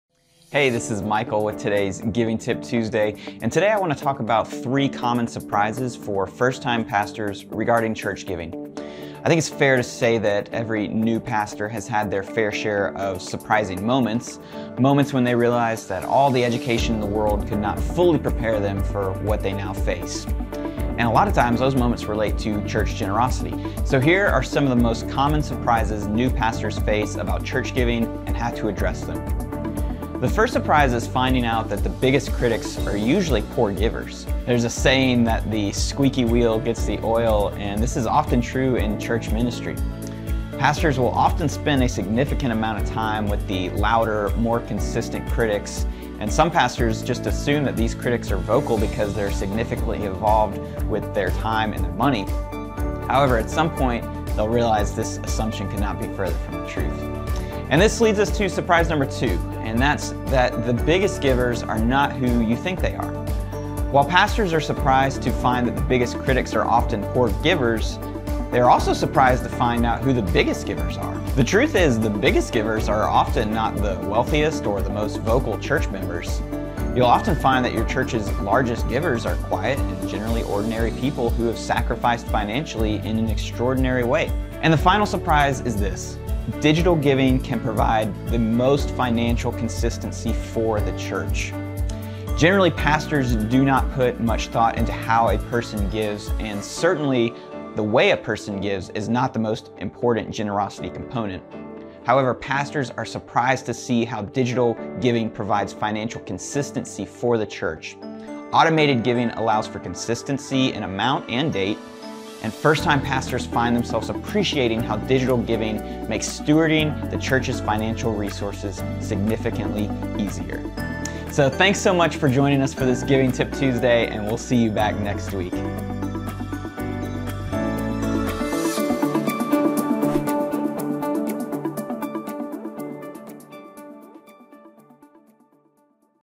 3 Common Surprises for First-Time Pastors on Church Giving | Giving Tip Tuesday, Episode 21
For this week’s Giving Tip Tuesday, Michael shares 3 common surprises first-time pastors face when it comes to church giving.